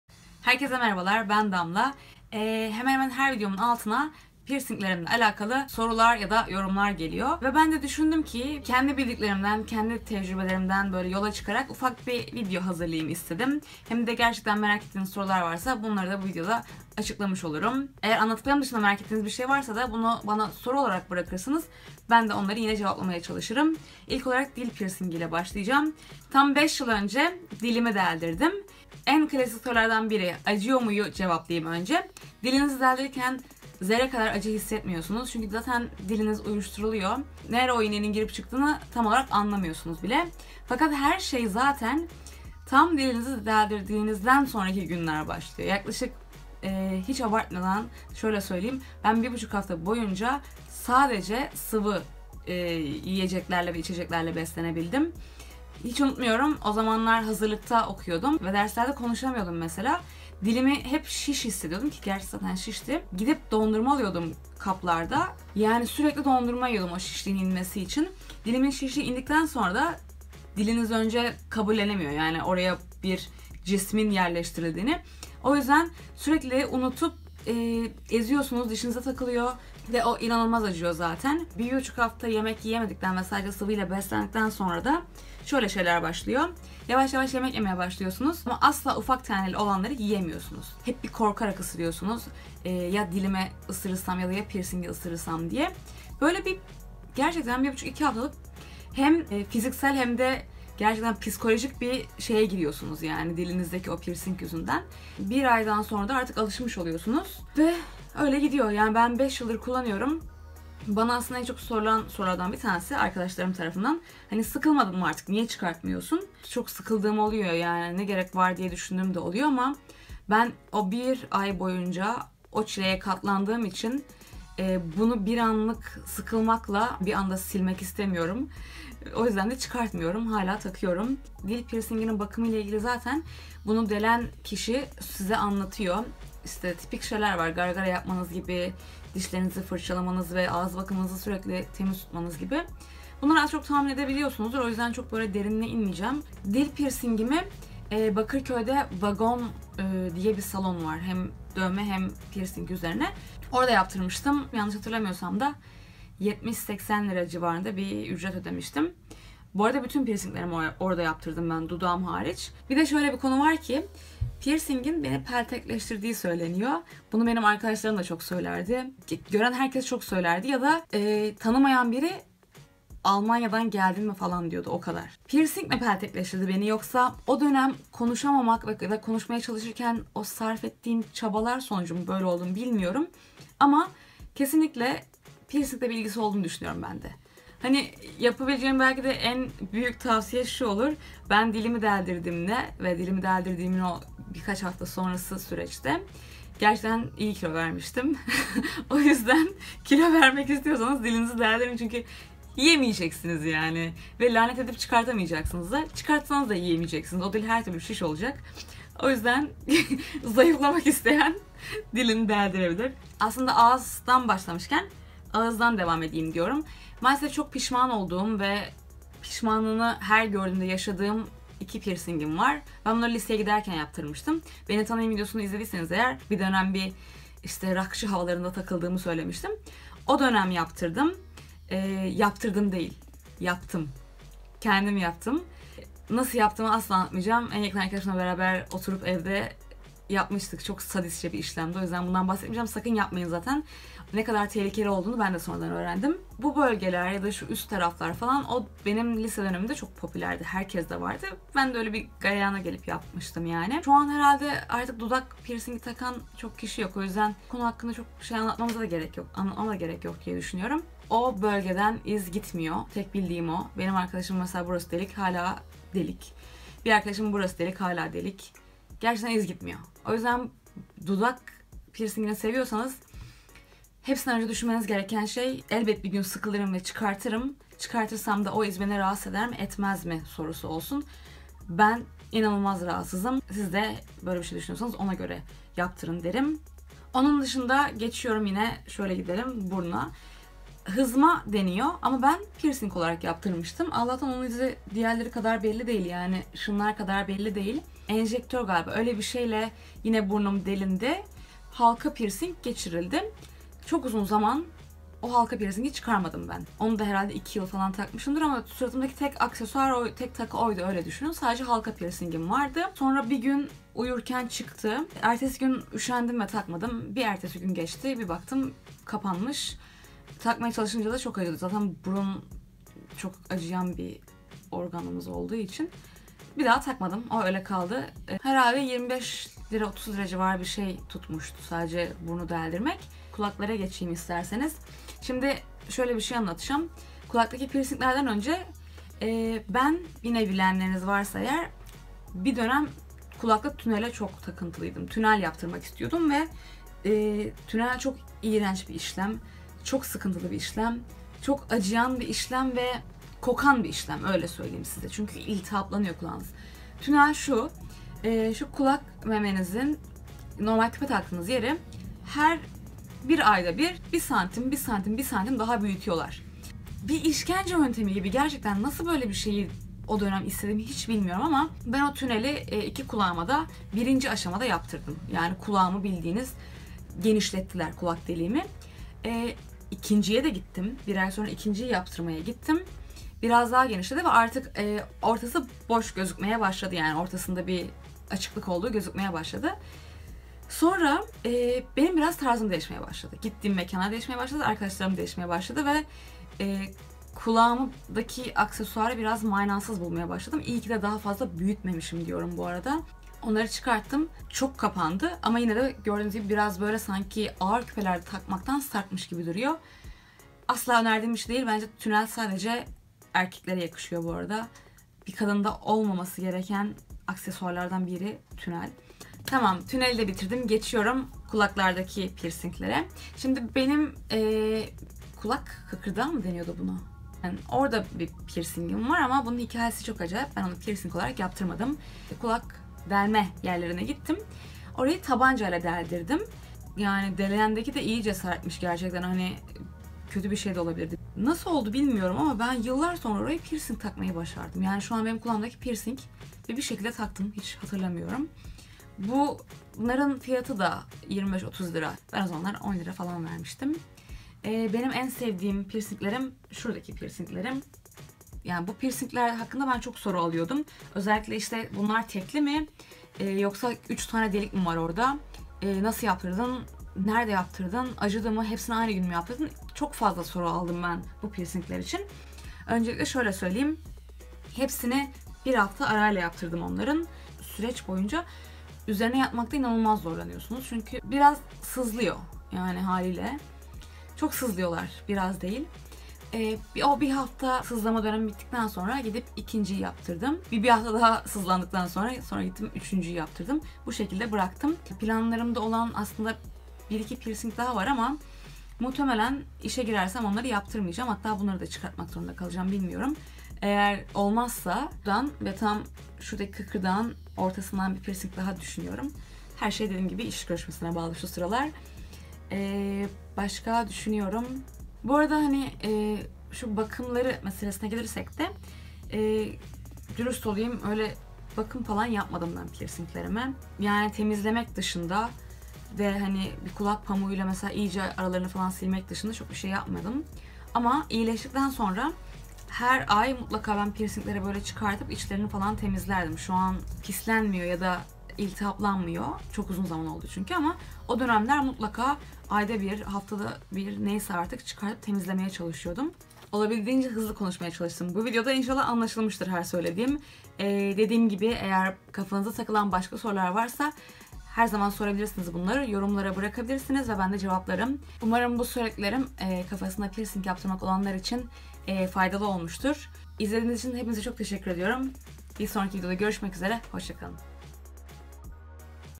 Piercinglerim | Merak Edilen Sorular & Ne, Nerede, Ne Kadar?
Herkese merhaba! Piercing yerleri, süreci, bakımı, fiyatı gibi konulara değinerek merak ettiğiniz soruları cevaplamaya çalıştım. Umarım yardımı dokunur :)

Hoşçakalın!